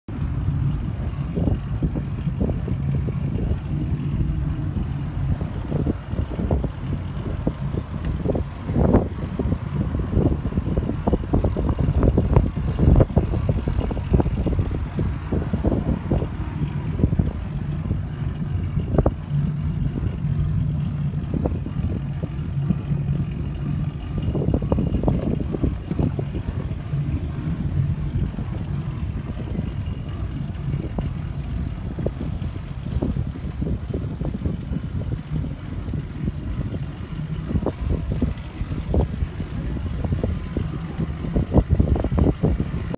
In giro per Stoccolma in ... risciò
Sono una specie  di risciò bianco coperto con pedalata assistita e si vedono dappertutto ; molto utili in caso di pioggia !